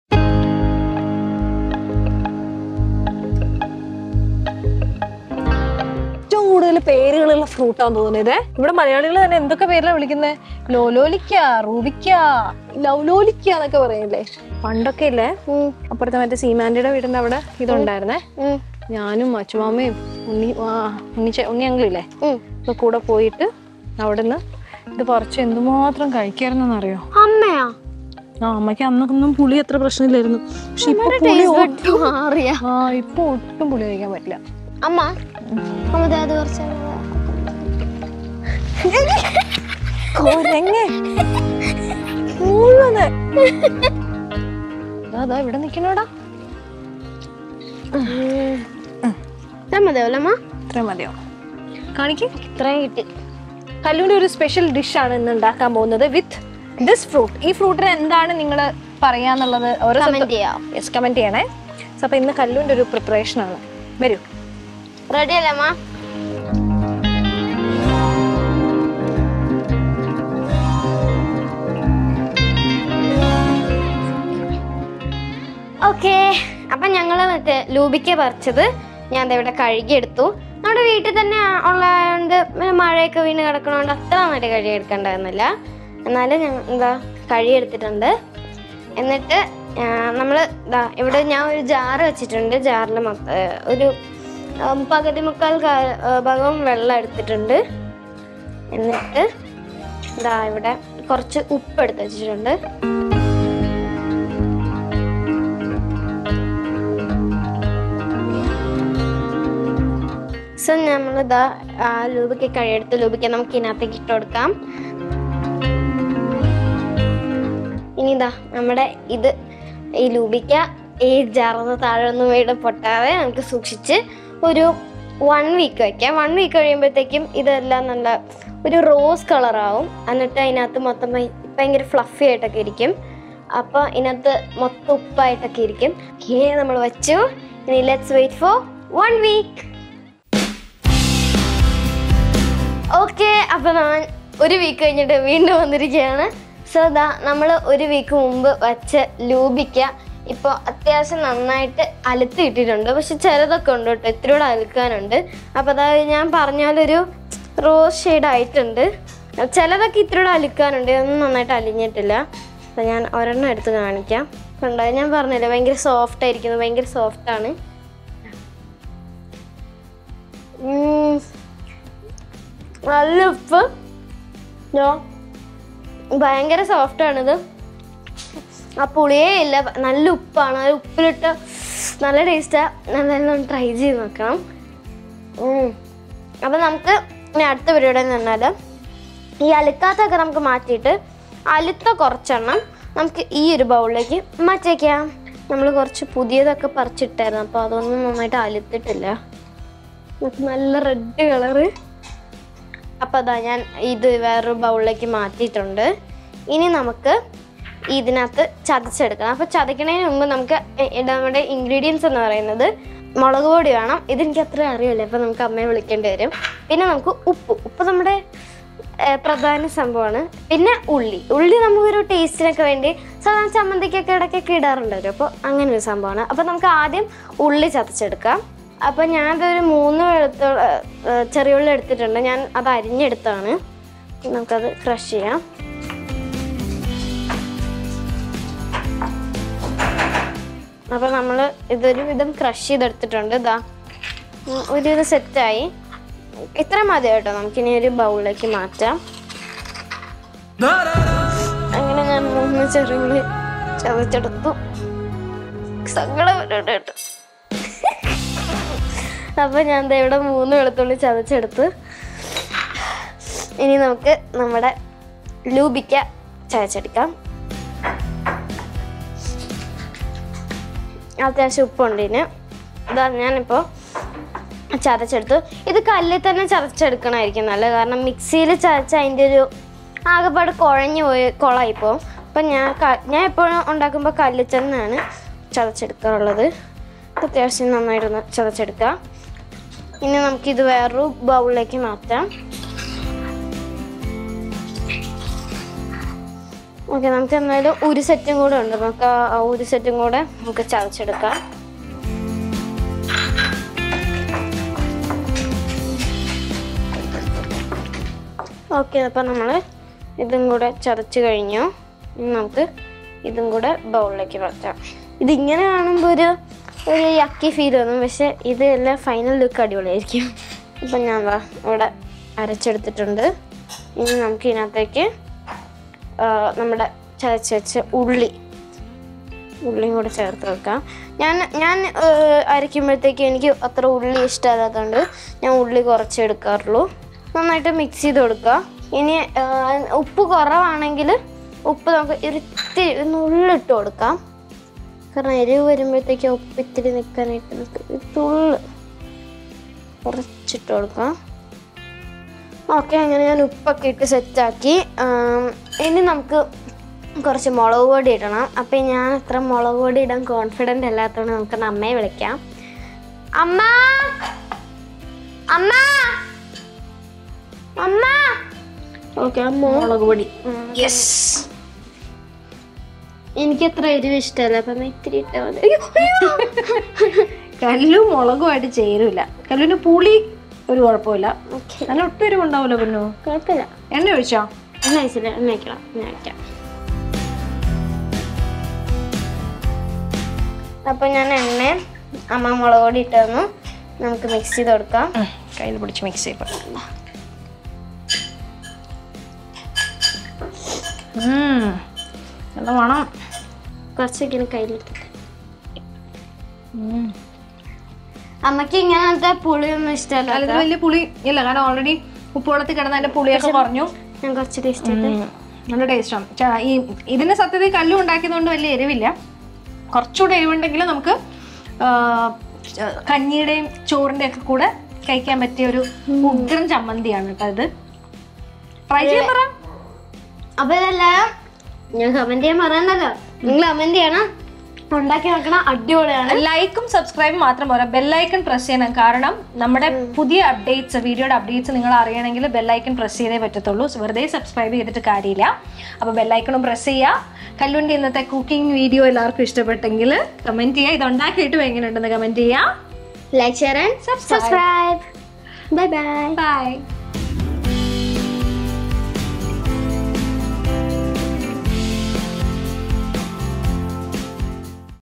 കല്ലുവിന്റെ പാചകം!! | Life Stories with Gayathri Arun
Dive into the world of Flacourtia jangomas with Kallus!